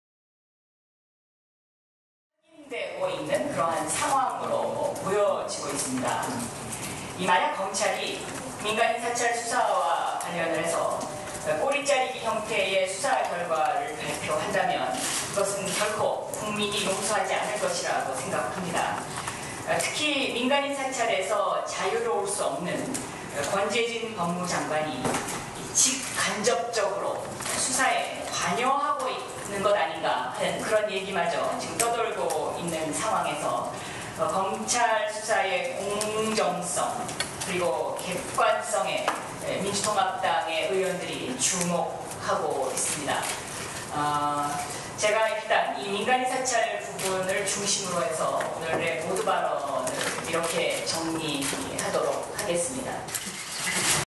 [경향신문]박영선 "검찰, 민간인 사찰 수사 꼬리자르기 국민이 용서않을 것"
1일 국회 민주통합당 원내대표실에서 MB새누리정권 부정부패청산국민위원회가 열렸다.

1차회의에서 박영선 위원장이 모두발언에서 "검찰의 민간인 사찰과 관련 꼬리자르기 수사결과를 발표한다면 국민이 용서하지 않을 것"이라 말한 뒤 "민간인 사찰에서 자유로울 수 없는 권재진 법무장관이 직간접적으로 수사에 관여하고 있다는 얘기가 있다"는 의혹을 제기했다.

〈강윤중 기자〉